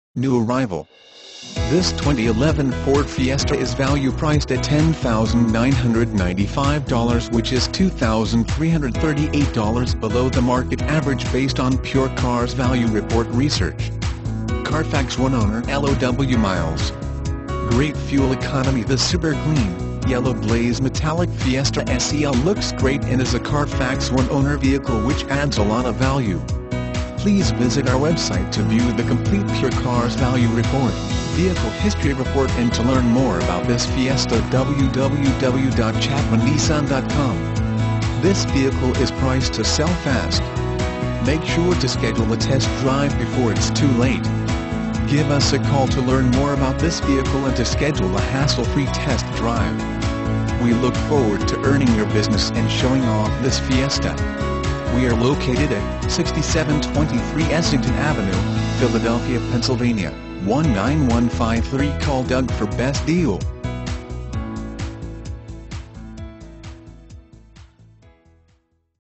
2011 FORD FIESTA Philadelphia, PA N24230A
2011 FORD FIESTA Philadelphia, PA
Stock# N24230A

NEW ARRIVAL! This 2011 Ford Fiesta is value priced at $10,995 which is $2,338 below the market average based on PureCars Value Report research! -CARFAX ONE OWNER- -LOW MILES!- - GREAT FUEL ECONOMY- This super clean, Yellow Blaze Metallic Fiesta SEL looks great and is a Carfax One Owner vehicle which adds a lot of value! Give us a call to learn more about this vehicle and to schedule a hassle free test drive! We look forward to earning your business and showing off this Fiesta! We are located at: 6723 Essington Ave, Philadelphia, PA 19153 Call Doug for best deal!